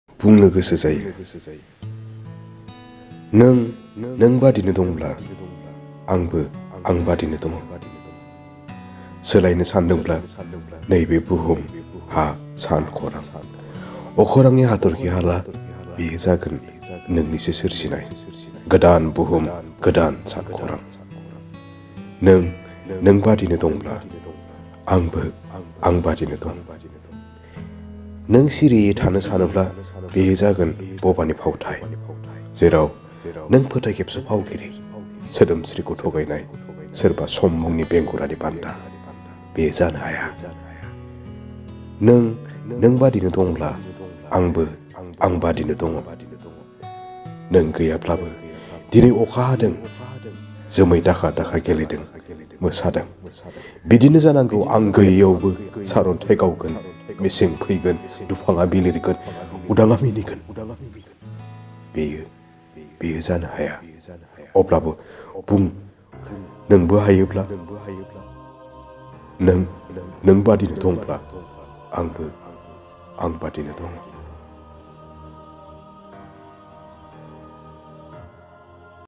Bungnw Gwsw Jayw || बुंनो गोसो जायो || Bodo Poem#2
Title: बुंनो गोसो जायो
'Bungnw Gwsw Jayw' a Bodo Poem
Written & Reading by Ripen Boro.
Content No.2

Source:
Baidi Gabgwnang Simang(Book)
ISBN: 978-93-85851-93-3
Published: 2021
Publisher: KITAP SAMALAYA,
Panbazar, Ghy-1